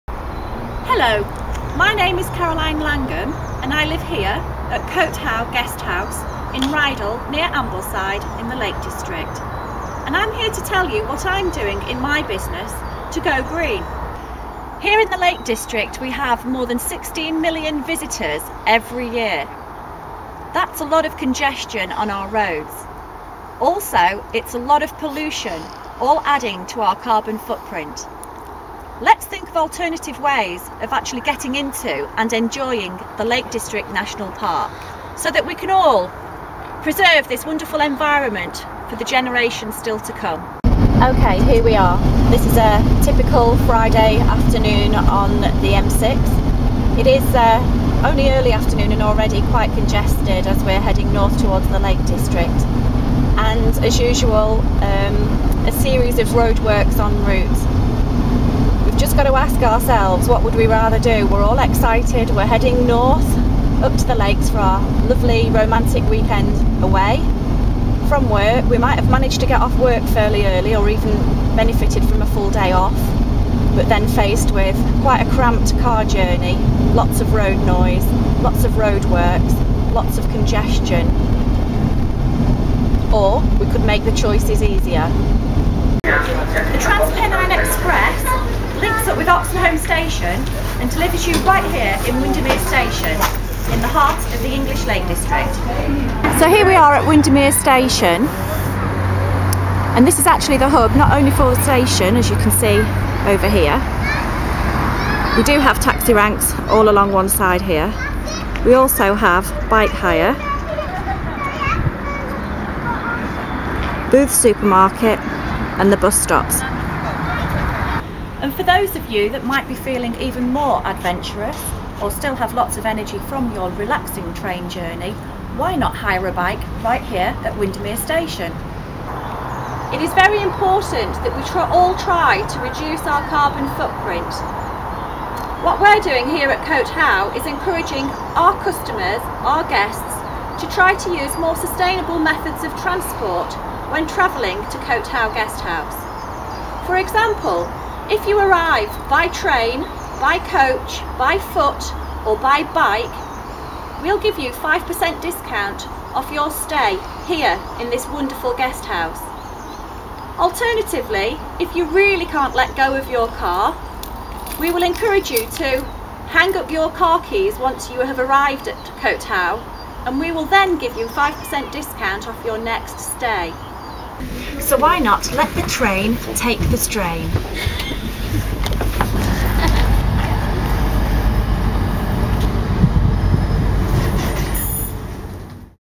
Caroline Langham Cote How organic bed and breakfast.mov
Caroline langham from Cote How organic bed and breakfast near ambleside talks about how she is trying to get customers to go green